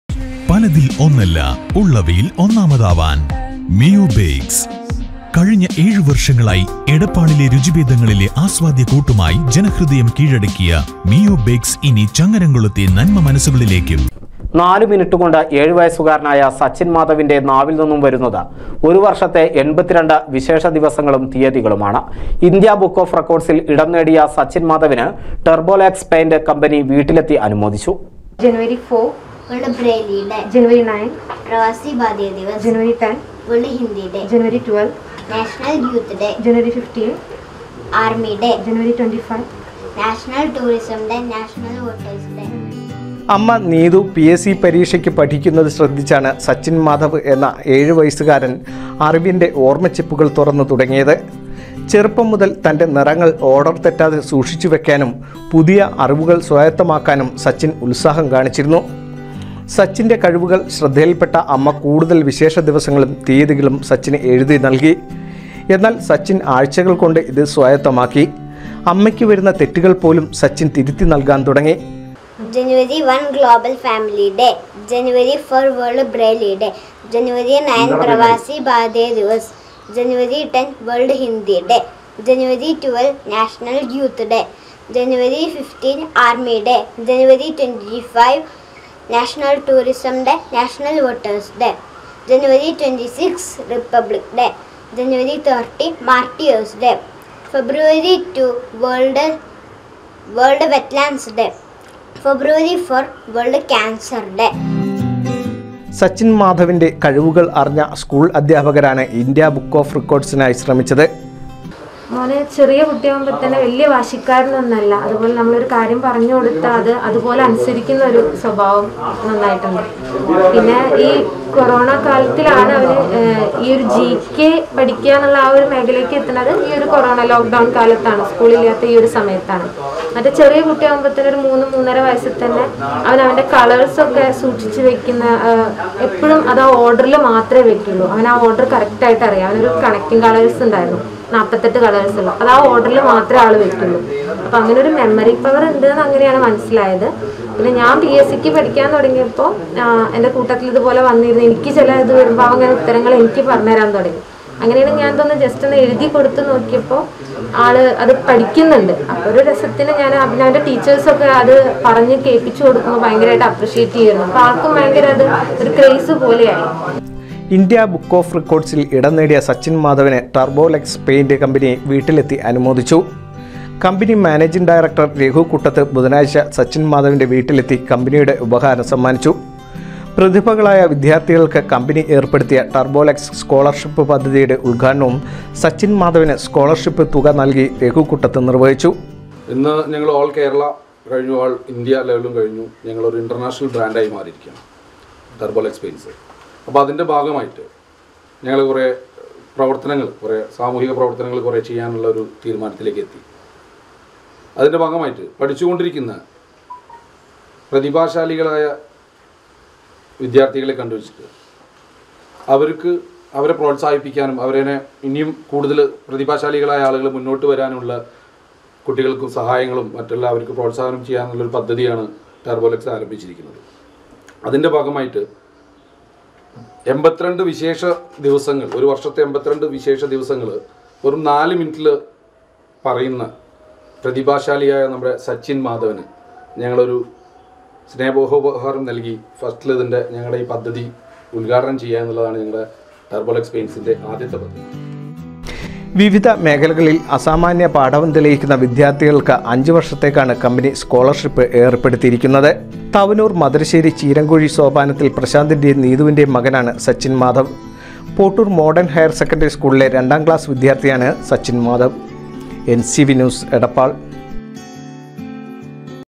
നാലു മിനിറ്റ് കൊണ്ട് 7 വയസുകാരനായ സച്ചിൻമാധവിൻ്റെ നാവിൽ ഒരുവർഷത്തെ 82 വിശേഷ ദിവസങ്ങളും തീയതികളുമാണ്
നാലു മിനിറ്റ് കൊണ്ട് 7 വയസുകാരനായ സച്ചിൻ മാധവിൻ്റെ നാവിൽ  ഒരുവർഷത്തെ 82 വിശേഷ ദിവസങ്ങളും തീയതികളുമാണ്. ഇന്ത്യ ബുക്ക്ഓഫ് റെക്കോർഡ്സിൽ ഇടം നേടിയ സച്ചിൻ മാധവിന്  ടെര്‍ബോലക്സ് പെയിന്റ് കമ്പനി വീട്ടിലെത്തി അനുമോദിച്ചു